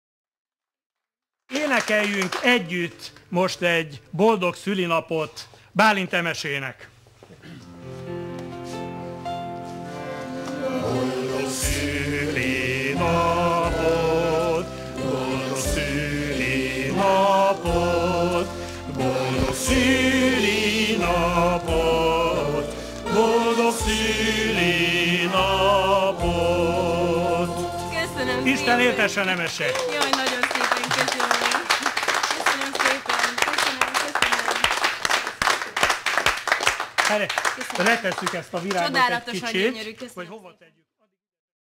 Bálint Emese énekes, harmonikás előadó, színésznő köszöntése élő adásban
A harmonikás - énekes Bálint Emesét köszönti Poór Péter táncdalénekes.
Emily - singer, accordion performer, actress Birthday Celebration on Hungarian TV by artist Peter Poór.